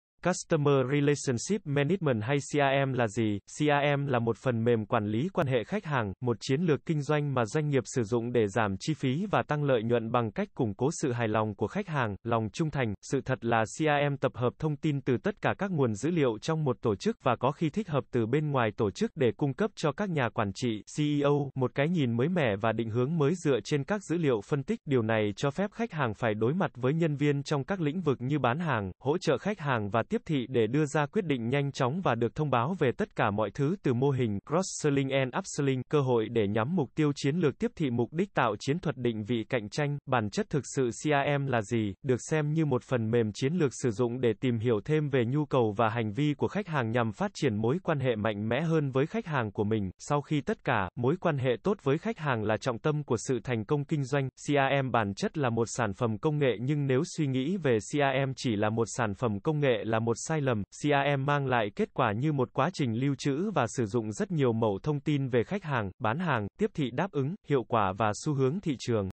CRM là gì? Những ý nghĩa của CRM - Nghialagi.org
Customer Relationship Management hay CRM là gì? CRM Là một phần mềm quản lý quan hệ khách hàng, một chiến lược kinh doanh mà doanh nghiệp sử dụng để giảm chi phí và tăng lợi nhuận bằng cách củng cố sự hài lòng của khách hàng, lòng trung thành. Sự thật là CRM tập hợp thông tin từ tất cả các nguồn dữ liệu trong một tổ chức (và có khi thích hợp từ bên ngoài tổ chức) để cung cấp cho các nhà quản trị (CEO) một cái nhìn mới mẻ và định hướng mới dựa trên các dữ liệu phân tích. Điều này cho phép khách hàng phải đối mặt với nhân viên trong các lĩnh vực như bán hàng, hỗ trợ khách hàng và tiếp thị để đưa ra quyết định nhanh chóng và được thông báo về tất cả mọi thứ từ mô hình "Cross-selling and upselling" cơ hội để nhắm mục tiêu chiến lược tiếp thị mục đích tạo chiến thuật định vị cạnh tranh.

Bản chất thực sự CRM là gì?

Được xem như một phần mềm chiến lược sử dụng để tìm hiểu thêm về nhu cầu và hành vi của khách hàng nhằm phát triển mối quan hệ mạnh mẽ hơn với khách hàng của mình. Sau khi tất cả, mối quan hệ tốt với khách hàng là trọng tâm của sự thành công kinh doanh. CRM bản chất là một sản phầm công nghệ nhưng nếu suy nghĩ về CRM chỉ là một sản phẩm công nghệ là một sai lầm. CRM mang lại kết quả như một quá trình  lưu trữ và sử dụng rất nhiều mẩu thông tin về khách hàng, bán hàng, tiếp thị đáp ứng, hiệu quả và xu hướng thị trường.

Địa chỉ: 999 Lê Trọng Tấn, Tân Phú, Hồ Chí Minh